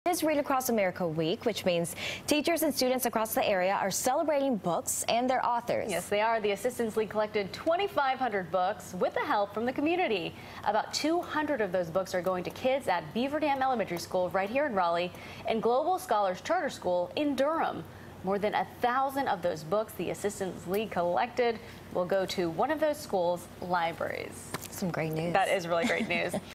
Celebrating 'Read Across America Week'
Books are being donated to two schools in the Triangle.